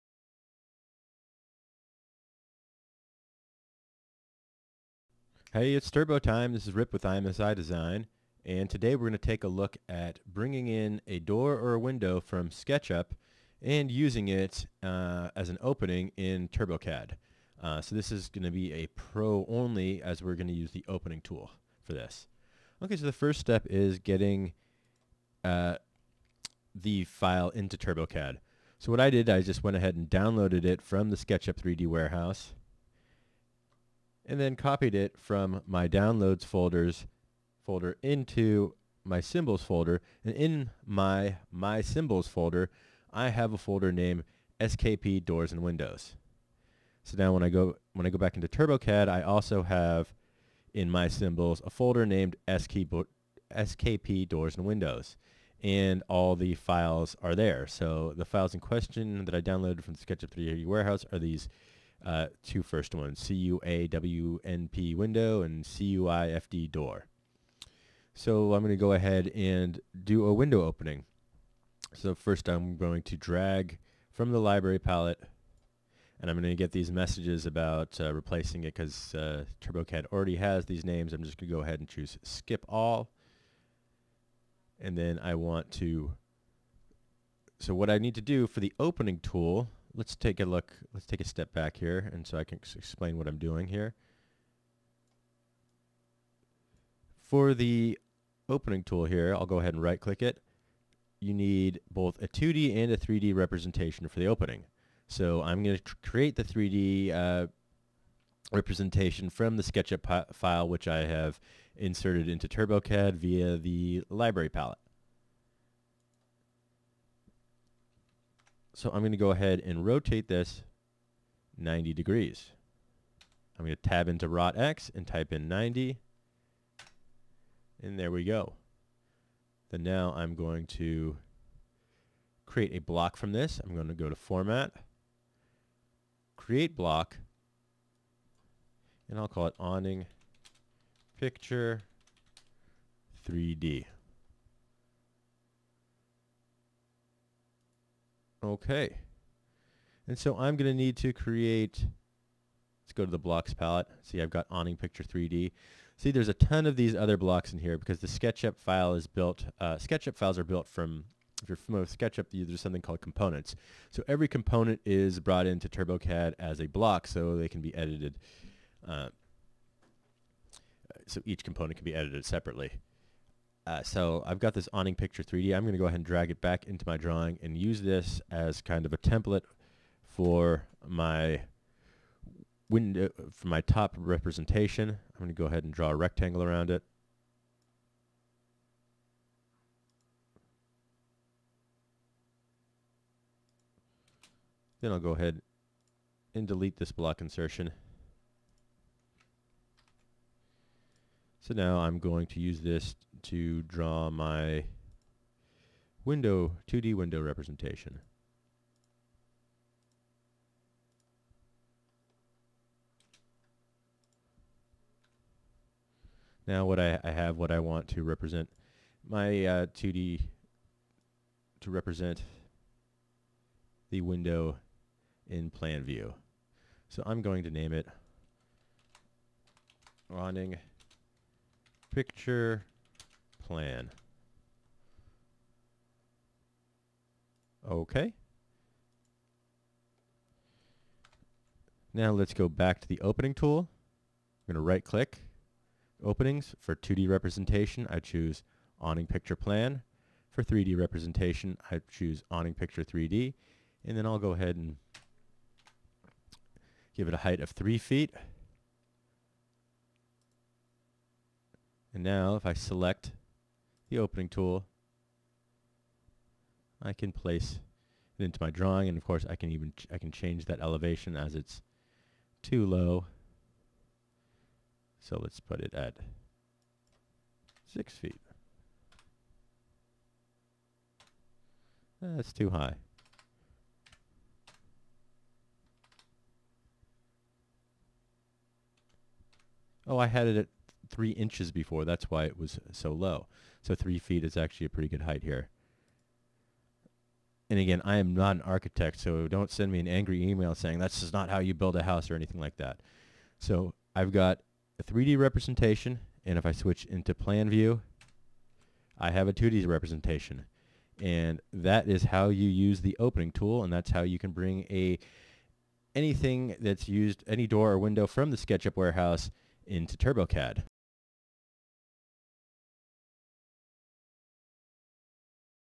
Using a Window from the Sketchup Warehouse in TurboCAD
This video shows how to use a window from the Sketchup 3D Warehouse in TC using the opening tool.